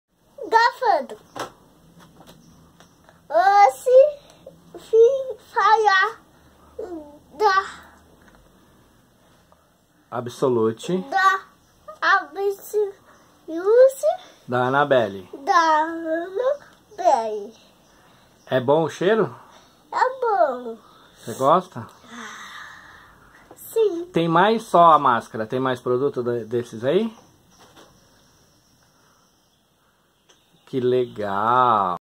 Kit home Care Absolut Anabelle.
O Kit Treat Hair Absolut foi desenvolvido exclusivamente para um novo conceito de beleza,que é tratar os fios com a mais alta qualidade.Para isso foi desenvolvido um produto rico em ativos hidratantes e polimeros nobres especiais que unidos reconstroem os fios deixando-os com um sensorial perfeito e uma fragrancia extremamente agradavel.